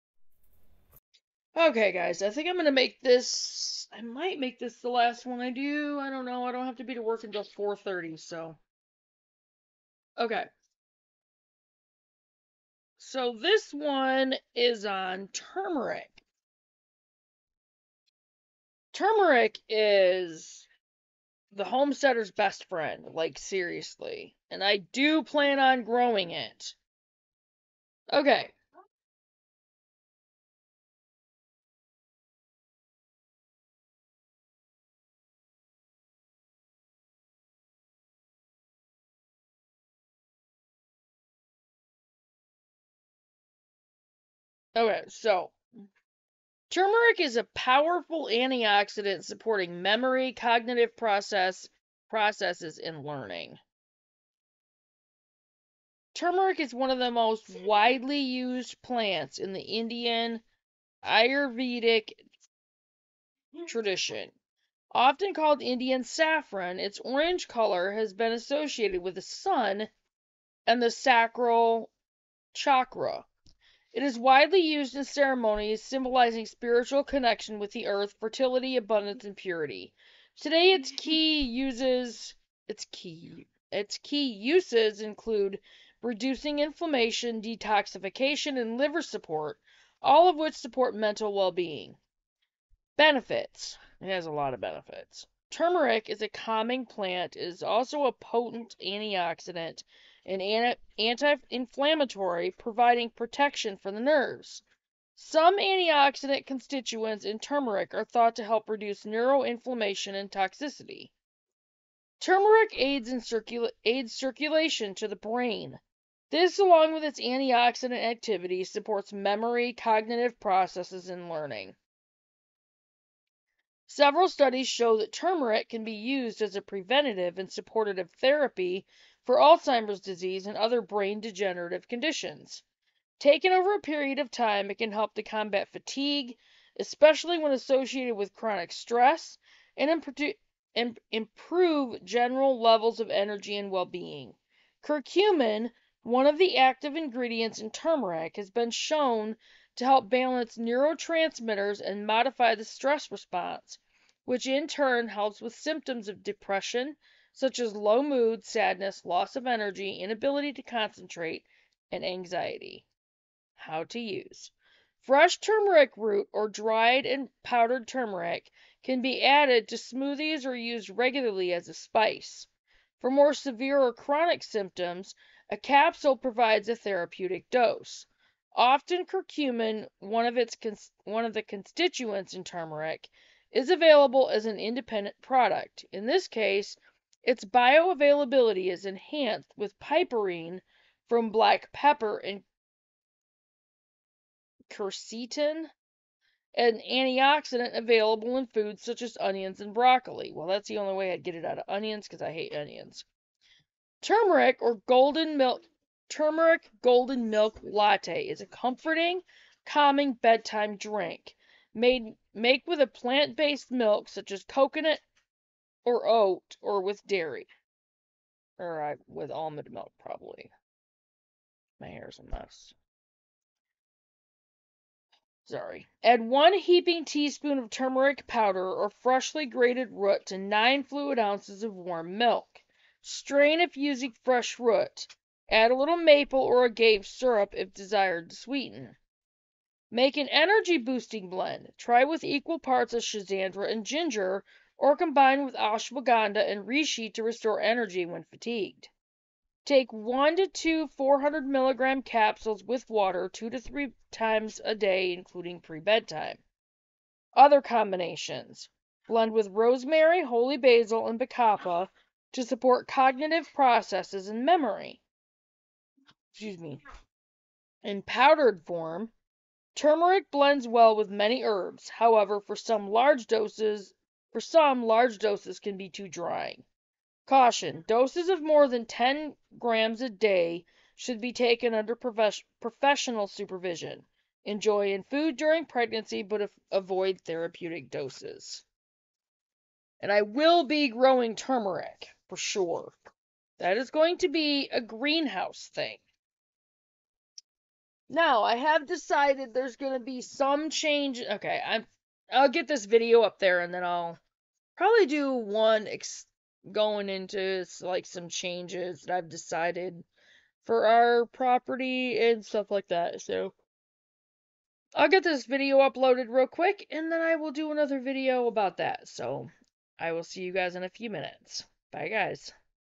Turmeric - Herb - How to Use
Turmeric is powerfully antioxidant, supporting memory, cognitive processes, and learning. It's key uses include reducing inflammation, detoxification, and liver support, all of which support mental well-being.